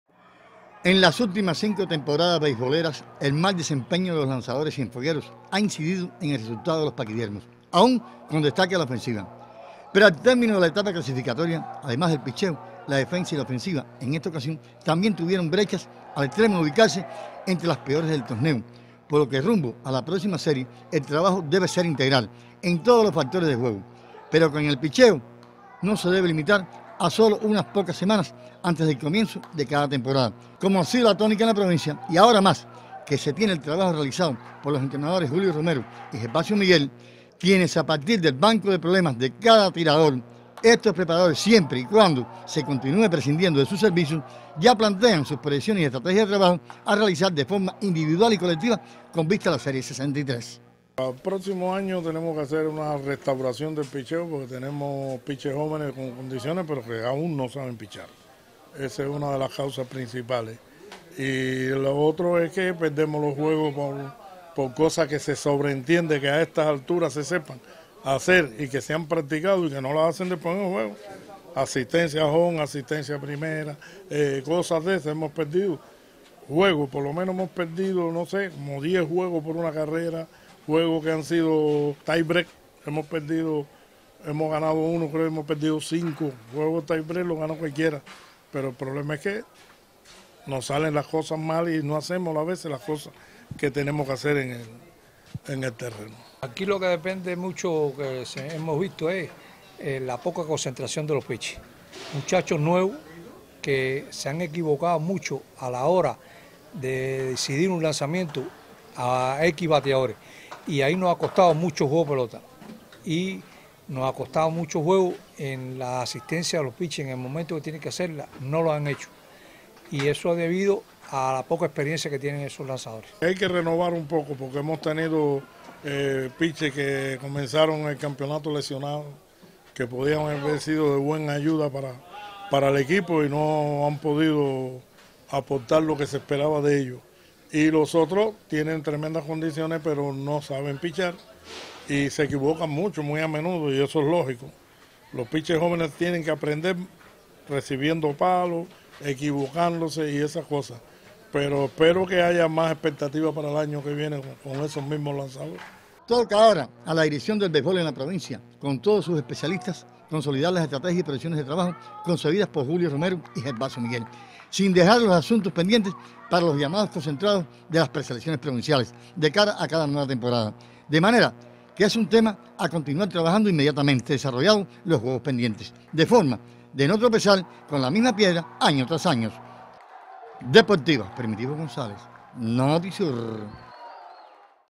Elefantes por perfeccionar aspectos imprescindibles en juego de béisbol
: Entre los aspectos a perfeccionar para un futuro próximo en la manda de los Elefantes de Cienfuegos se encuentran el control de los lanzadores y mayor repertorio en su desempeño, así como el cumplimiento del pensamiento técnico y táctico, preconcebido para cada momento